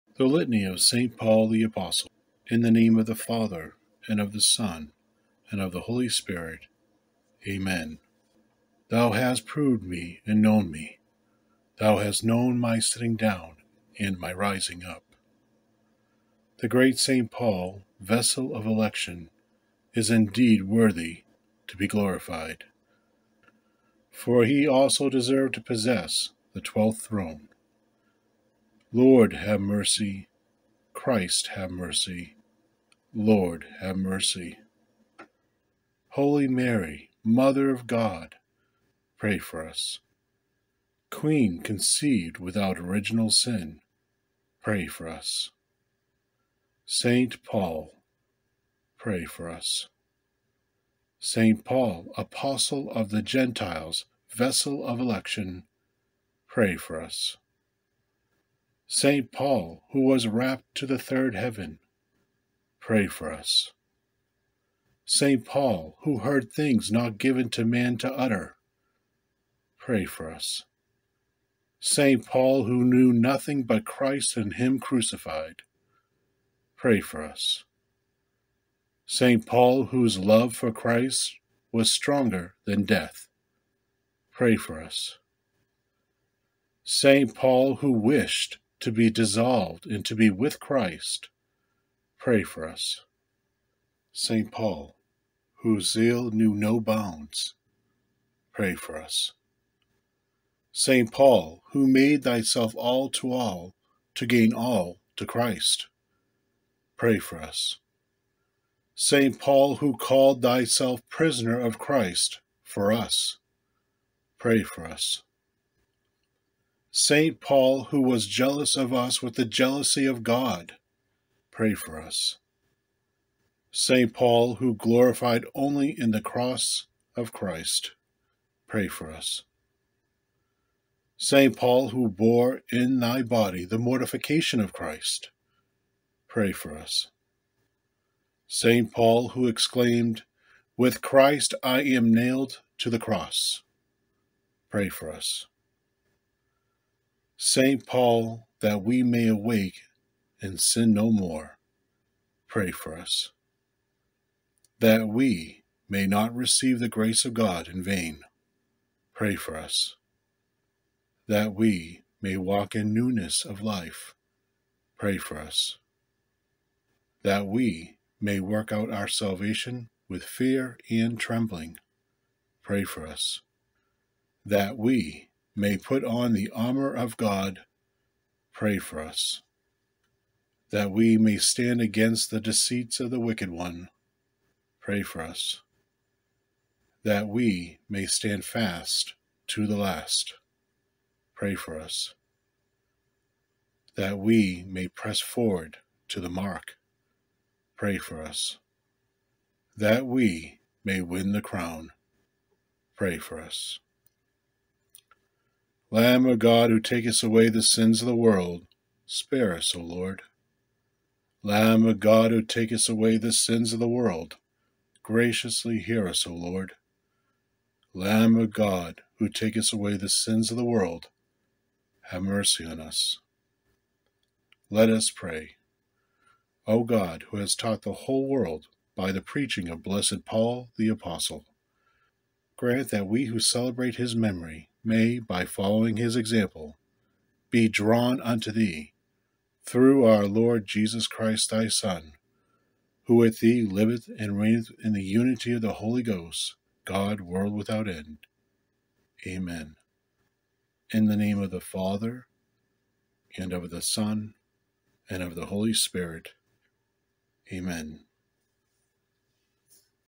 Prayer- Litany of St. Paul, Apostle of the Gentiles
St Paul was born 3 B.C.  at Tarsus, Cilicia (modern Turkey) as Saul.  He was a Jewish Talmudic student and Pharisee. He was of the tribe of Benjamin, and a pupil of Gamaliel.   Paul was a Tent-maker by trade. Saul the Jew hated and persecuted Christians (not yet called Catholics, which was later coined by St. Ignatius of Antioch in 98 A.D.) as heretical, even assisting in the stoning of Saint Stephen the Martyr. On his way to Damascus, Syria, to arrest another group of faithful, he was knocked to the ground, struck blind by a heavenly light, and given the message that in persecuting Christians, he was persecuting Christ.

Acts 9:3-5
"And as he went on his journey, it came to pass that he drew nigh to Damascus. And suddenly a light from heaven shined round about him. And falling on the ground, he heard a voice saying to him: Saul, Saul, why persecutest thou me? Who said: Who art thou, Lord? And he: I am Jesus whom thou persecutest."

The experience had a profound spiritual effect on him, causing his conversion to Christianity. He was baptized, changed his name to Paul to reflect his new persona, and began traveling, preaching, and teaching. He would become the greatest missionary for the Catholic Church. His letters to the churches he assisted in converting form a large percentage of the New Testament. He knew and worked with many of the earliest saints and fathers of the Church.

The Emperor Nero caused him to be beheaded on the same day that Peter was crucified. He was martyred in Rome, and therefore listed as Apostle to the Gentiles and  Martyr of the Catholic Church.

Feasts of St. Paul, Apostle to the Gentiles:
January 25 (celebration of his conversion)
February 10 (Saint Paul Shipwrecked)
June 29 (celebration of Saint Peter and Saint Paul, traditionally known as their date of martyrdom) *The Roman Rite of the Mass of June 29, the majority of the texts refer either to St Peter alone (Introit, Epistle, Alleluia, Gospel, Communion) or to Apostles generically, as in the Gradual “Thou shalt make them princes over all the earth.” The sole reference to St Paul is in the Collect, “O God, who hast consecrated this day by the martyrdom of Thy Apostles Peter and Paul, grant Thy Church to follow in all things the teaching of those through whom she first received the faith.” The Office is likewise dedicated almost entirely to St Peter. Therefore, the Feast is more known for its celebration of the First Pope, St. Peter. The following day, the whole of the Liturgy is dedicated to St Paul.
June 30 (Commemoration of St. Paul)
November 18 (feast of the dedication of the Basilicas of Peter and Paul)

Litany of St. Paul the Apostle of the Gentiles

Thou hast proved me and known me;
Thou hast known my sitting down and my rising up.

V. The great Saint Paul, vessel of election, is indeed worthy to be glorified,
R. For he also deserved to possess the twelfth throne.

V. Lord, have mercy.
R. Christ, have mercy.

Holy Mary, Mother of God, R. pray for us.
Queen conceived without Original Sin, R. pray for us.
Saint Paul, etc.
Apostle of the Gentiles, Vessel of election,
St. Paul, who wast rapt to the third heaven,
St. Paul, who heard things not given to man to utter,
St. Paul, who knew nothing but Christ and Him crucified,
St. Paul, whose love for Christ was stronger than death,
St. Paul, who wished to be dissolved and to be with Christ,
St. Paul, whose zeal knew no bounds,
St. Paul, who made thyself all to all, to gain all to Christ,
St. Paul, who called thyself prisoner of Christ for us,
St. Paul, who wast jealous of us with the jealousy of God,
St. Paul, who gloried only in the Cross of Christ,
St. Paul, who bore in thy body the mortification of Christ,
St. Paul, who exclaimed: "With Christ I am nailed to the cross!"
St. Paul, that we may awake and sin no more,
That we may not receive the grace of God in vain,
That we may walk in newness of life,
That we may work out our salvation with fear and trembling,
That we may put on the armor of God,
That we may stand against the deceits of the wicked one,
That we may stand fast to the last,
That we may press forward to the mark,
That we may win the crown,

Lamb of God, Who takest away the sins of the world,
R. Spare us, O Lord.
R. Graciously hear us, O Lord.
R. Have mercy on us.

 O God, Who hast taught the whole world by the preaching of blessed Paul the Apostle, grant that we who celebrate his memory may, by following his example, be drawn unto Thee, Through Our Lord Jesus Christ Thy Son, Who with Thee liveth and reigneth in the unity of the Holy Ghost, God, world without end.
R. Amen.